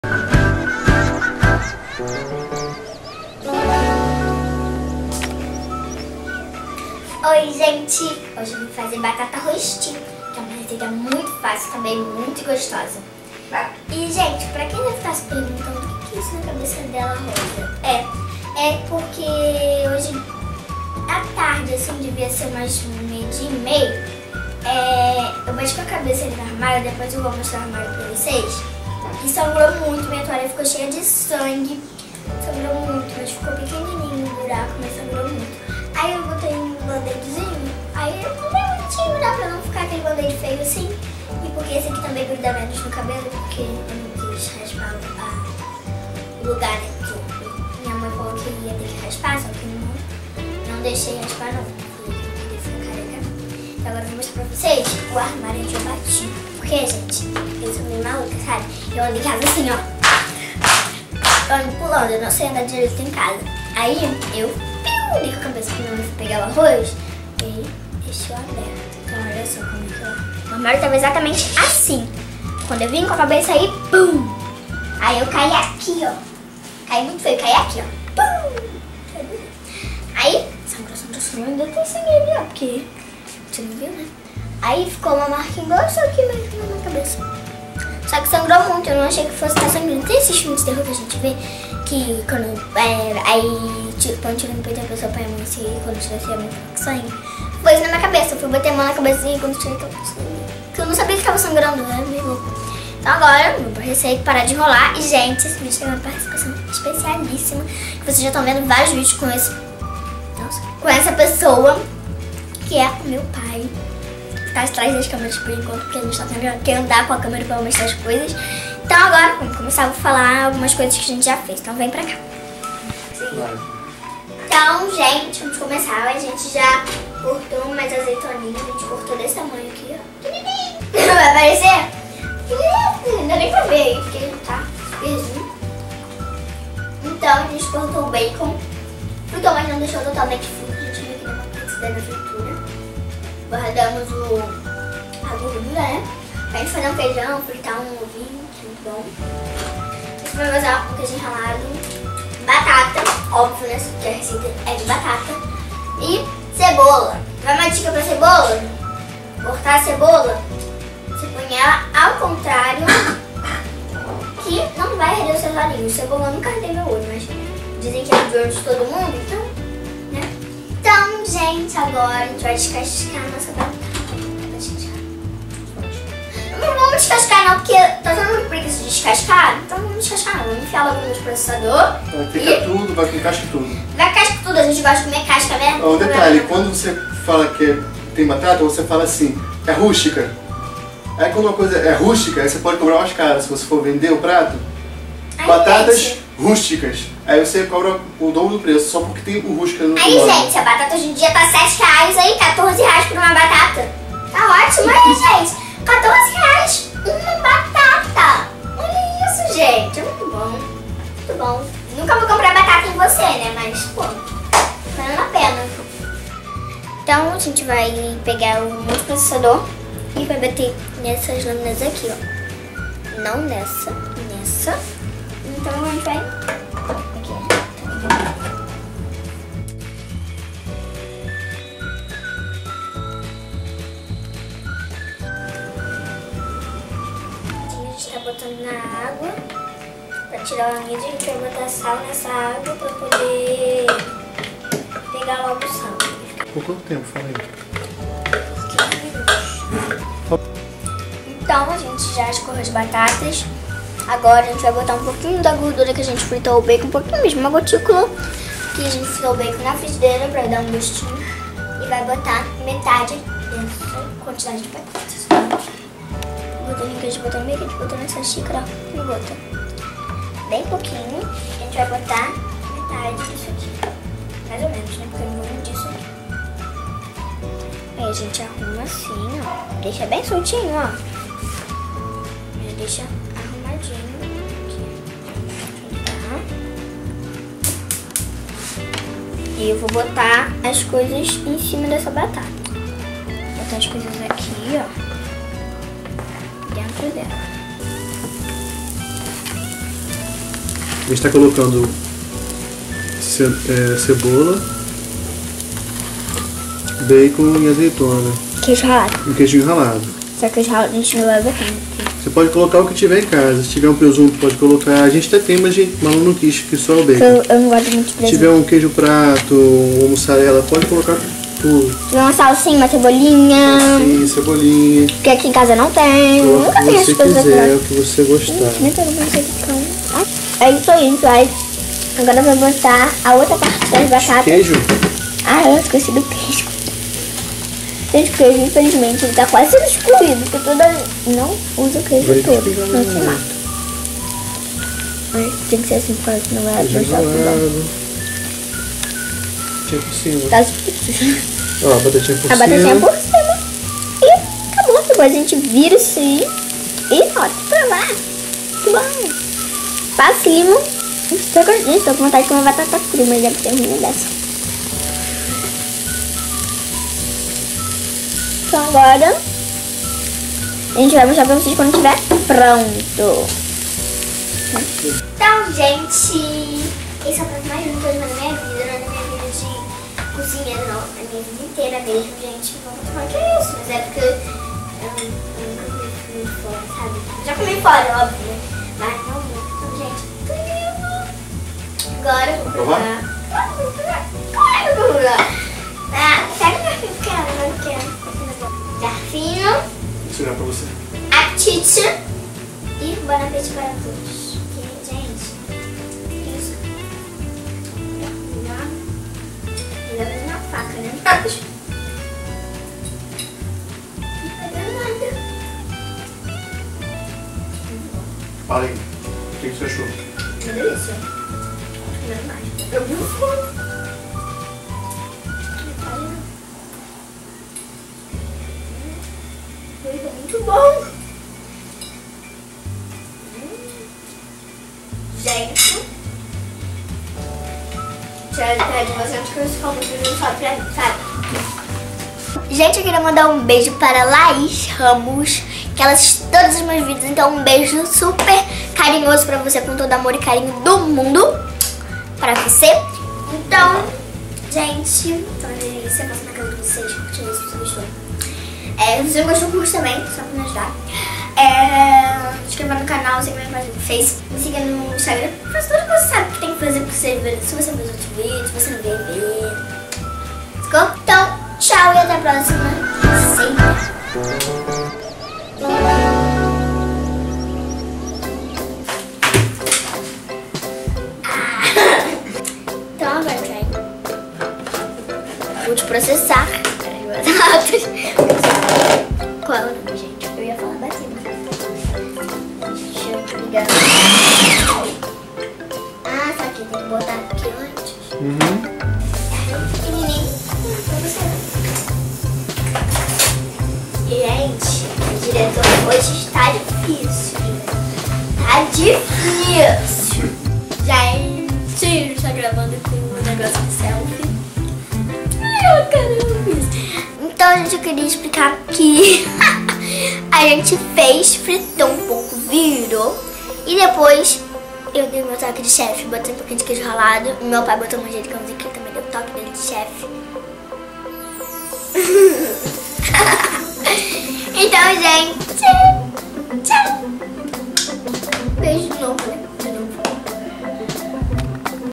Como Fazer Batata Rostie
Aprenda a fazer Batata Rostie e impressione seus amigos! Uma delícia super fácil de fazer!!!

Ingredientes:

-  Batata inglesa a gosto
- Cebola, azeitona, bacon, queijo e outros temperos a gosto

Forma de preparo: Assista ao Vídeo e aprenda! ¯\_(ツ)_/¯

Se gostou, não se esqueça de deixar seu "like"! ¯\_(ツ)_/¯¯\_(ツ)_/¯